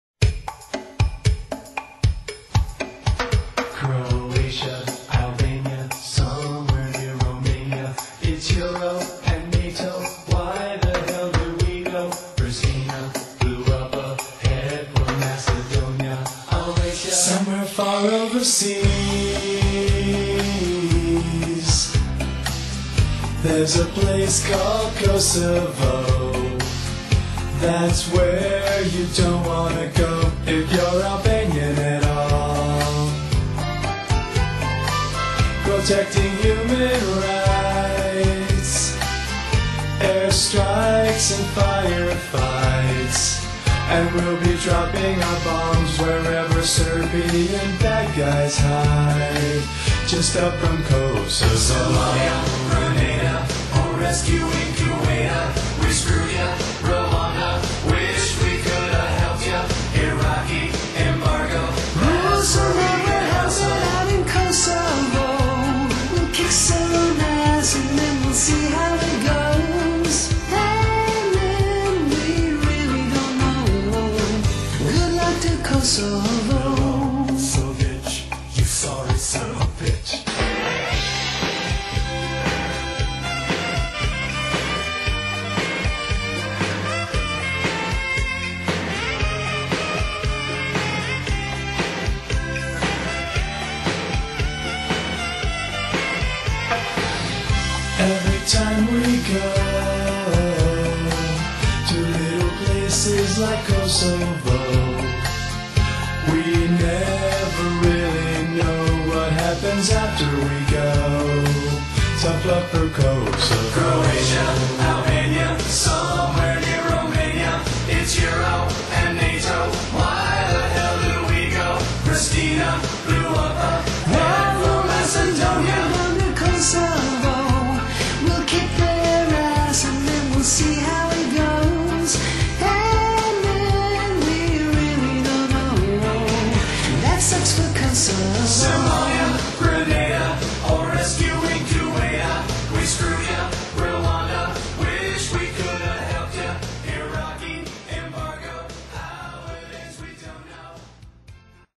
Kosovo Music Video
What do you get when you leave soldiers with too much time on thier hands, in the middle of nowhere with music equipment? This Funny Video!!!

You may want to turn up your volume a little bit cuz the music is kinda quiet.